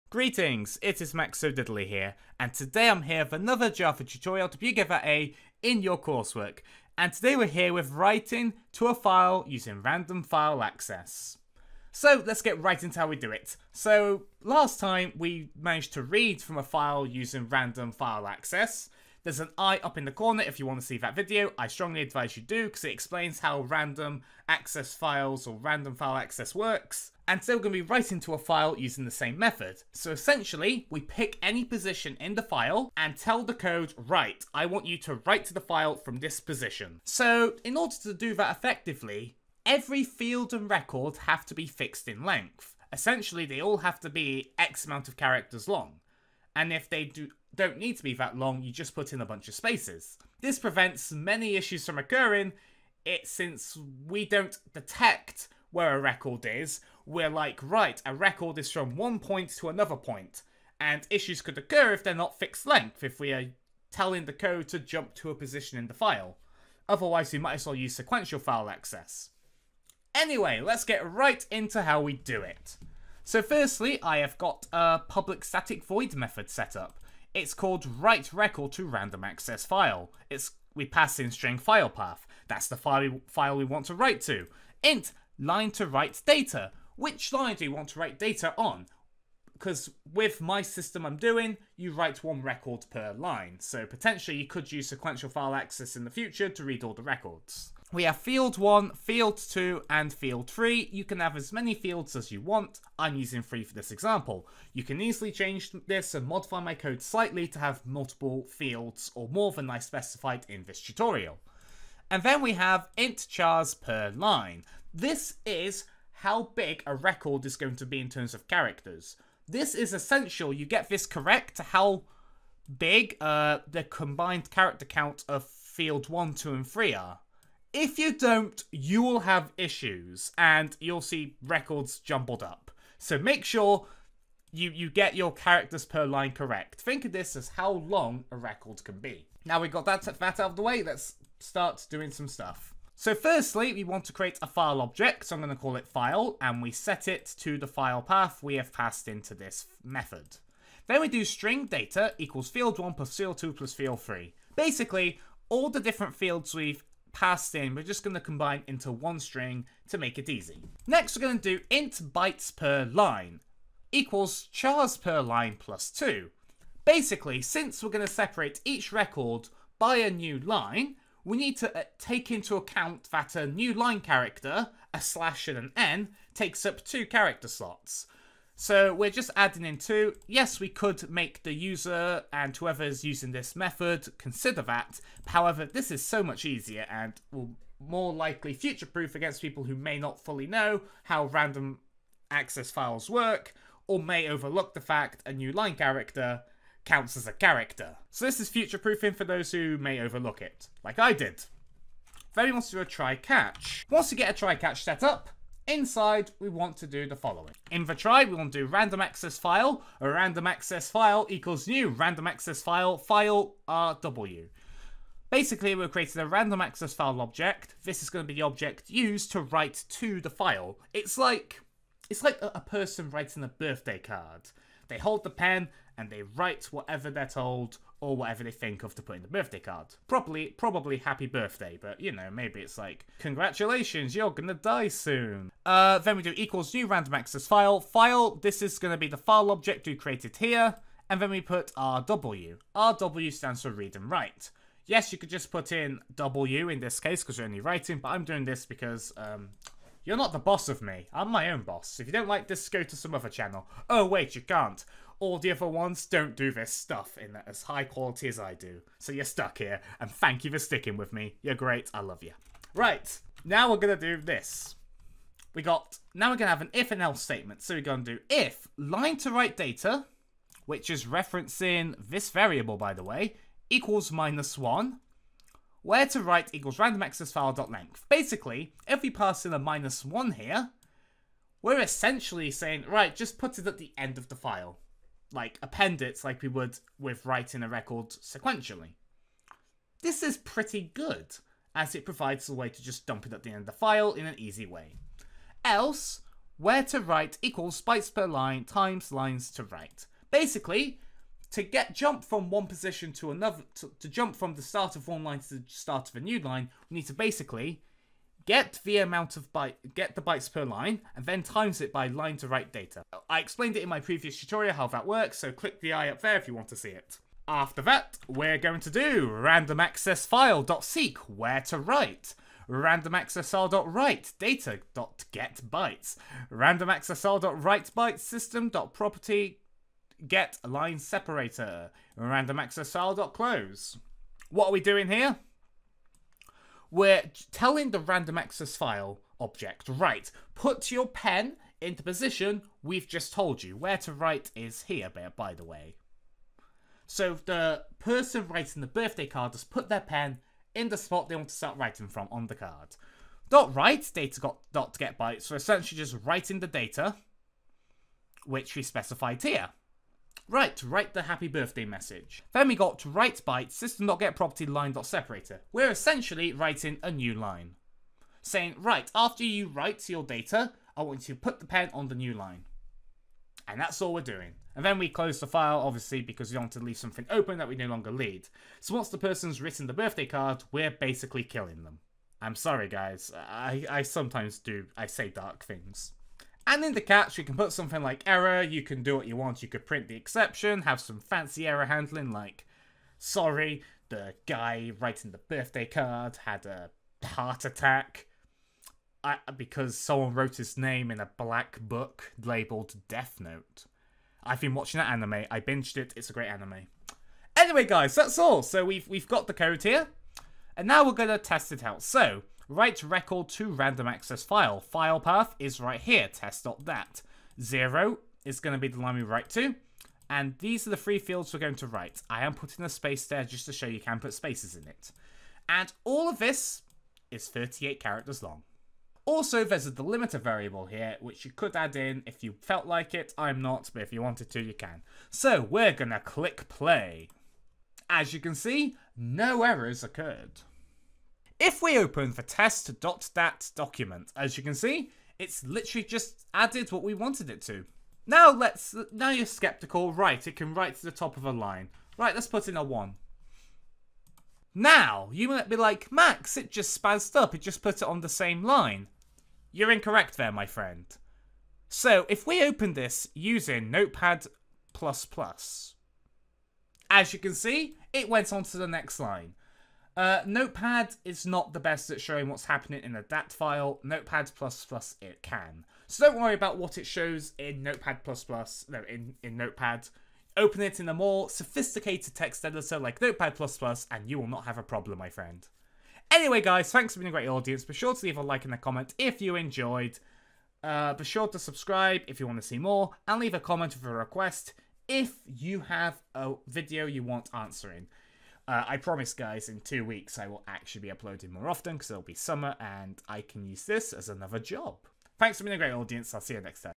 Write a record to a Random Access File using Java
Greetings, today I shall be showing you how to write to a random access file using Java. This is a follow up to my reading from a random access file video. Essentially, random access files allow us to jump to any position in a file and write from where we jump to. Very handy for performance but requires records and possibly fields to be fixed in length to avoid some issues that could arise.